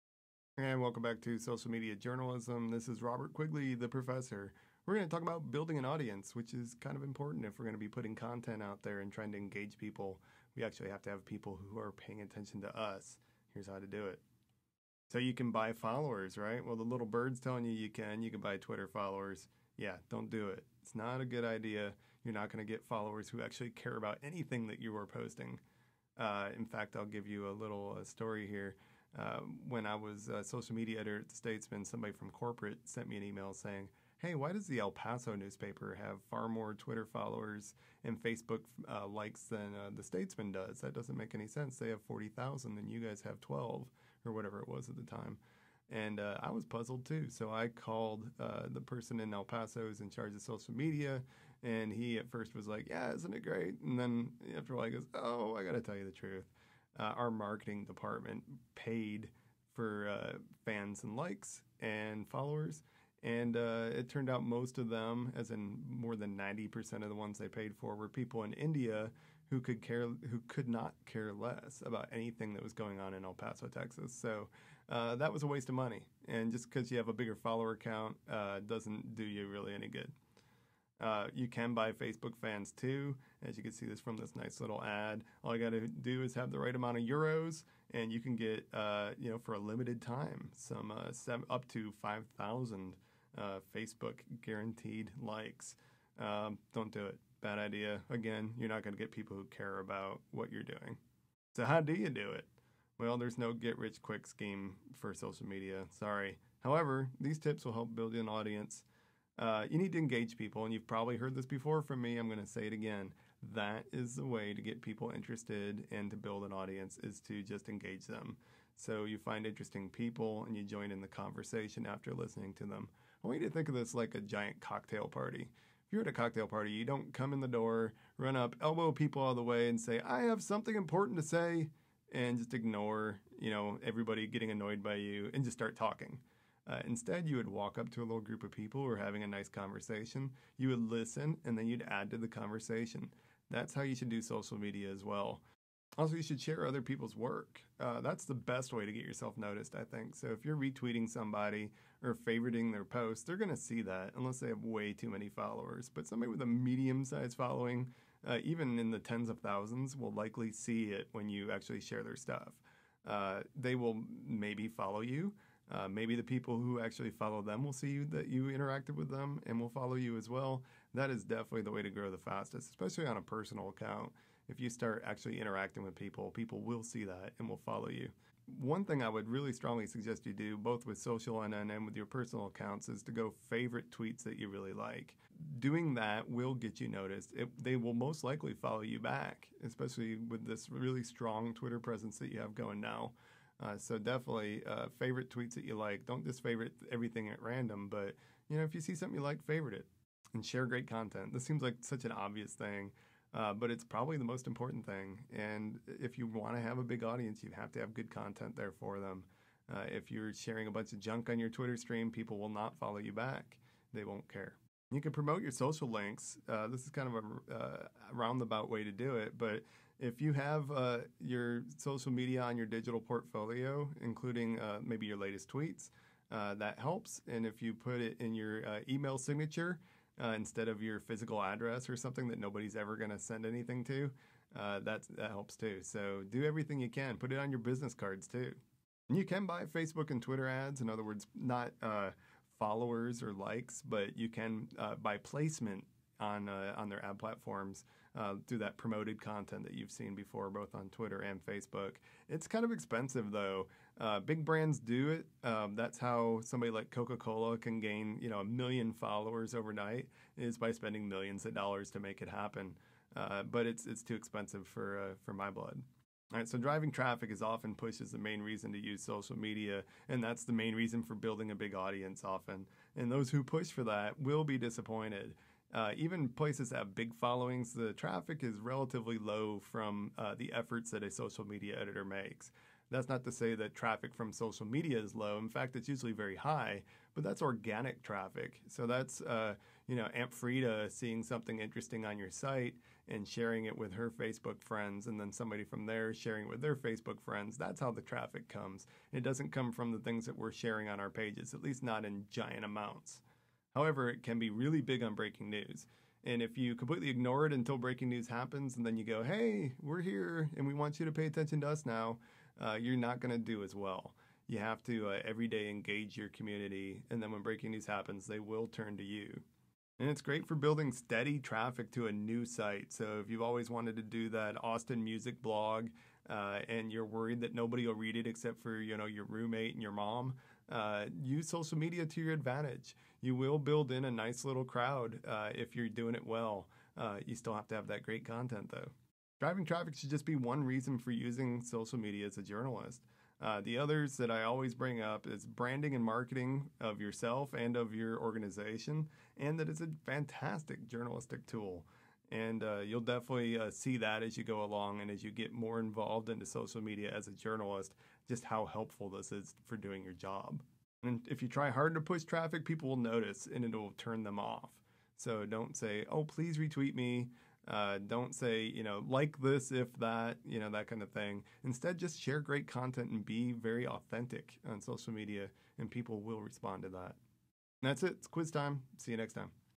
Building Audience: Social Media Journalism
Part of an online class by Robert Quigley for the University of Texas at Austin School of Journalism.